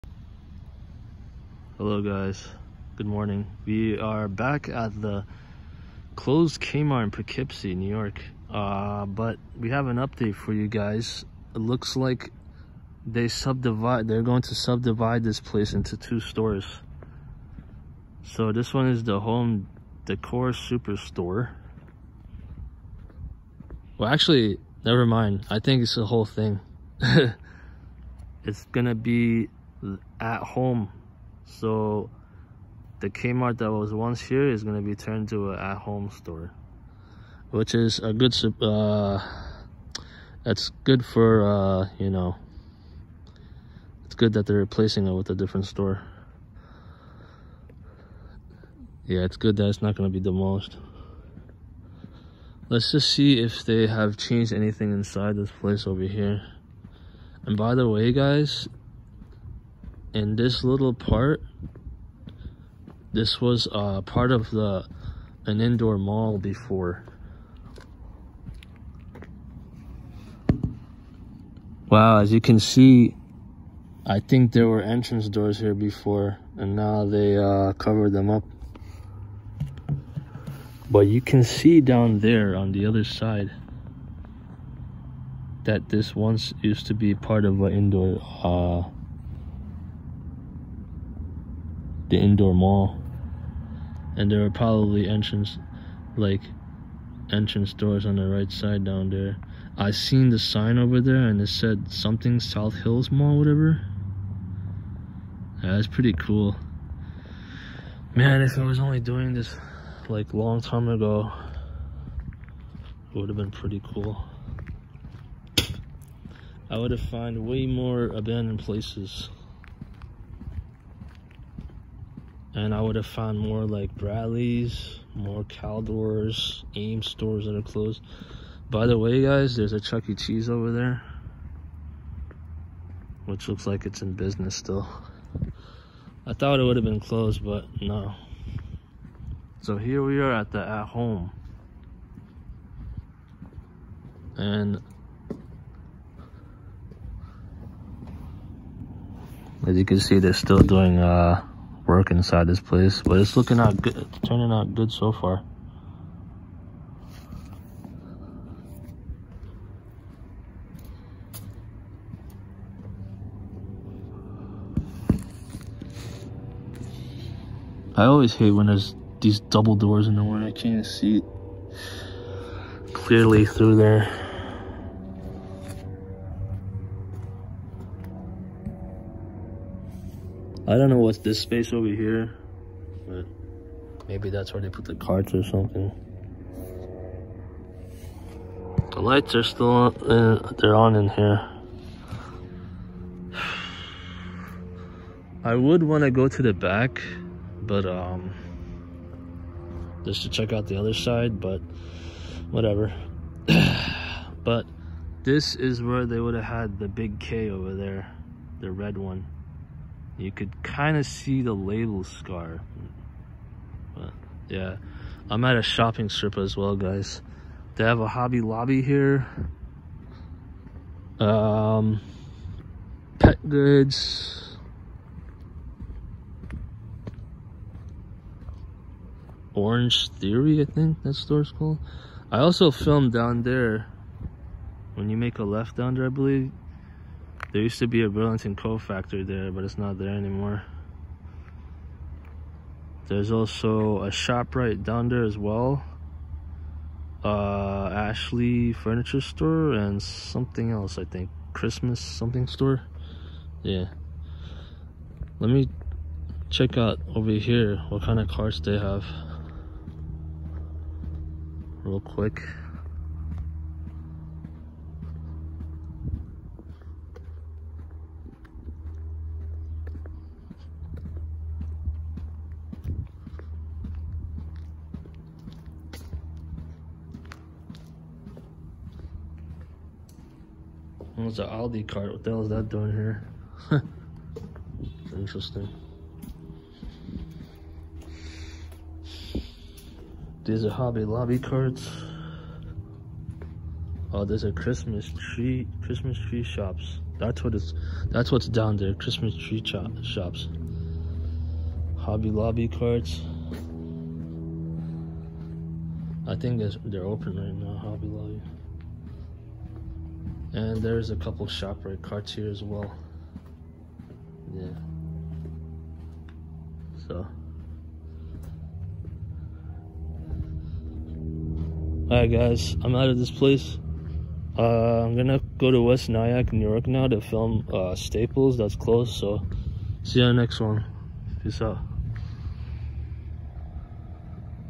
Revisit of Abandoned Kmart in Poughkeepsie (Soon to be At Home)
This Kmart location closed down in July 2016 and will be an At Home store this month of October 2021. The inside of the store is pretty much clean and all the lights are on, it just needs the merchandise, shelving and register counters and whatever else for it to be ready to be open to the public.